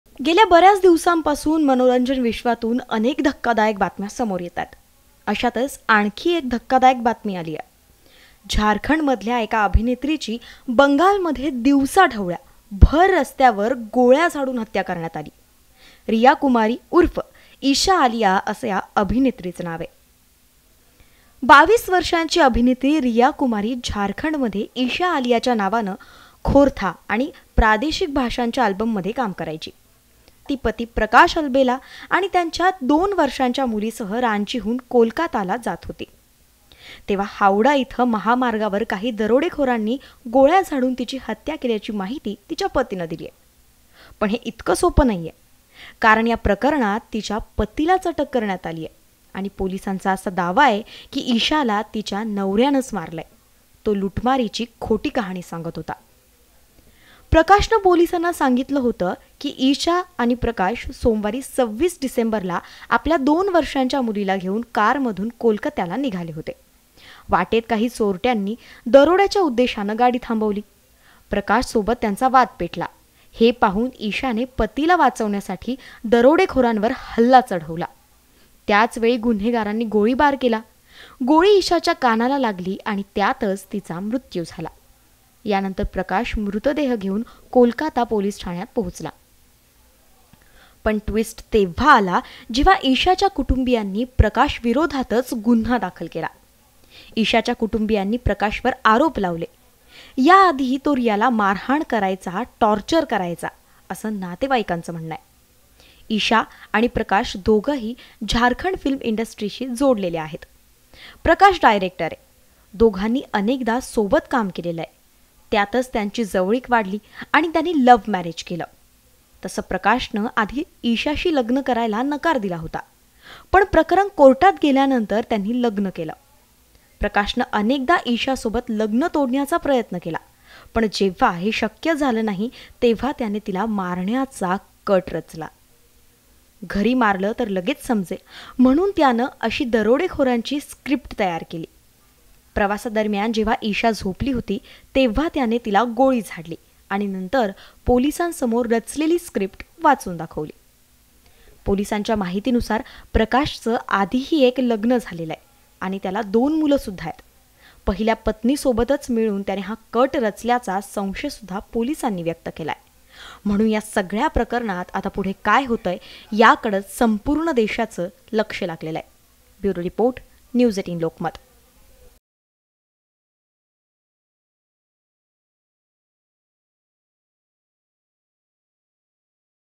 Isha Aliya Murder Case | पतीनेच केली अभिनेत्रीची हत्या; पोलिसांनी कशी केली गुन्ह्याची उकल?
Isha Aliya Murder case : झारखंडमधील एका अभिनेत्रीची बंगालमध्ये दिवसा ढवळ्या, भर रस्त्यावर गोळ्या झाडून हत्या करण्यात आली. रिया कुमारी उर्फ ​​ईशा आलिया असं या अभिनेत्रीचं नाव आहे. परंतु ही हत्या तिच्या पतीनेच केली असल्याचा पोलिसांना संशय आहे.